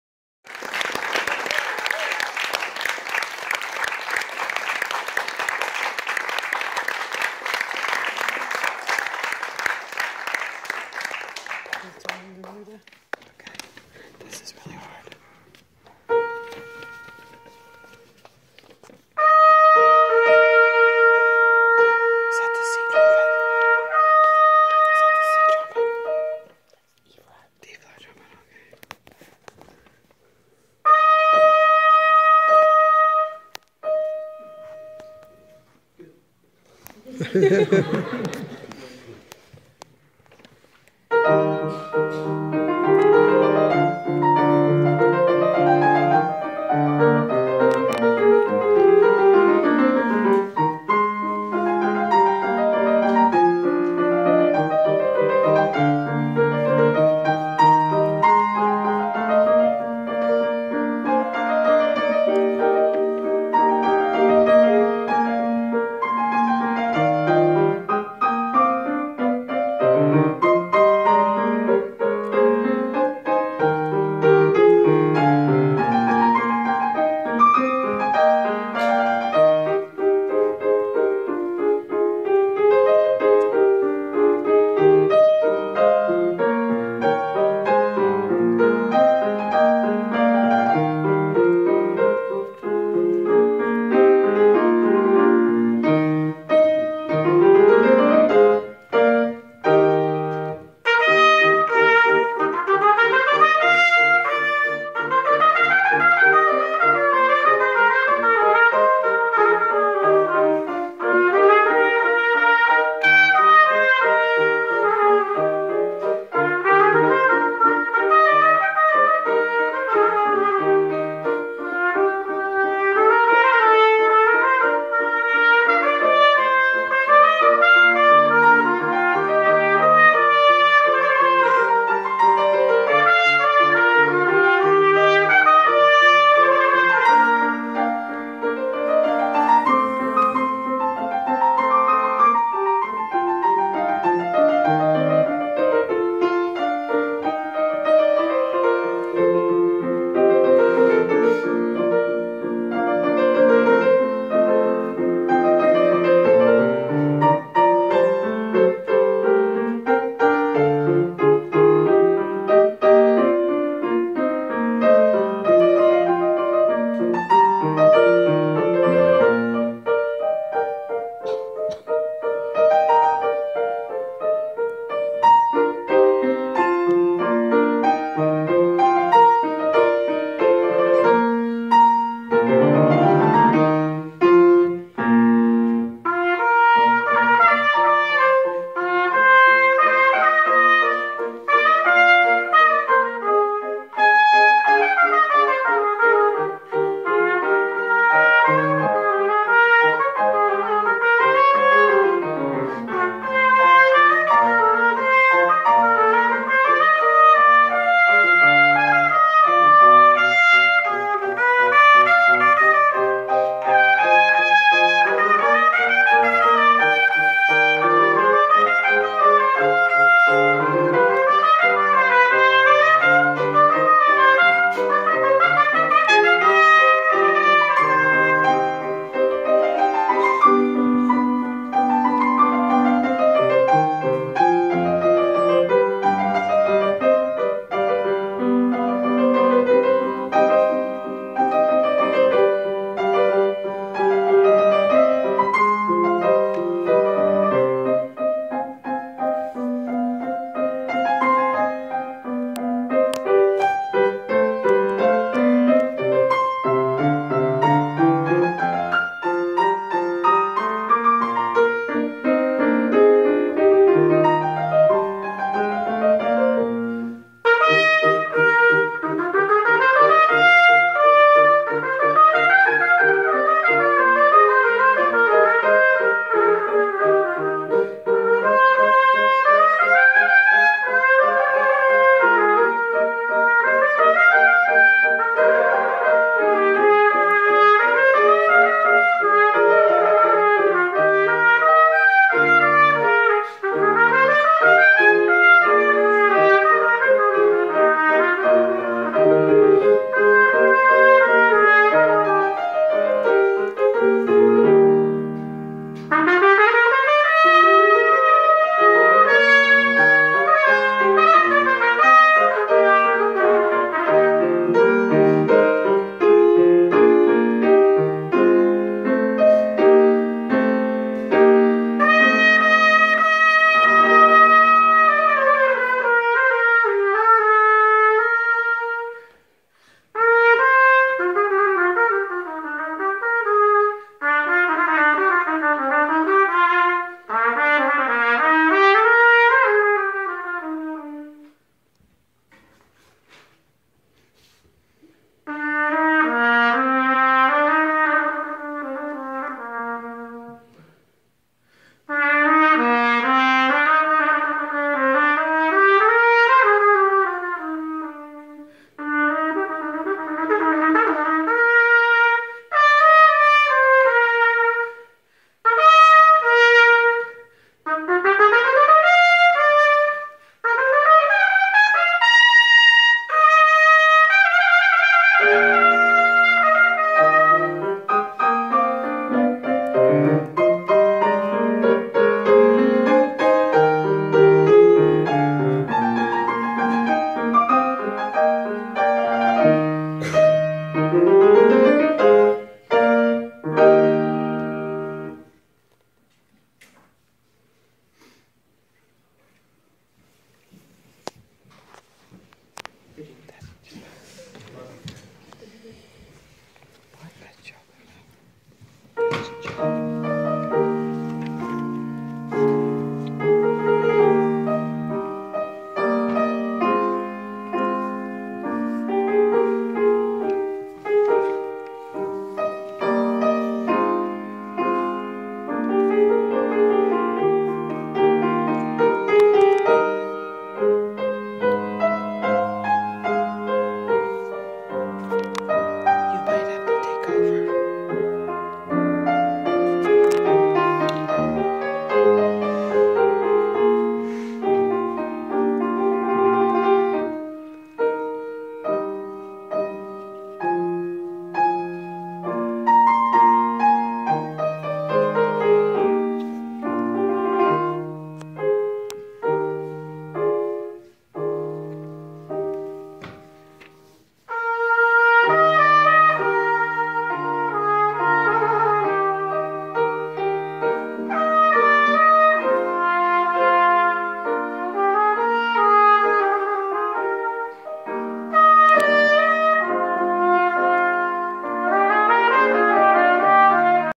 James V. Millen II, Trumpet. Juinor recital. May 2017, The Crane School of Music at SUNY Potsdam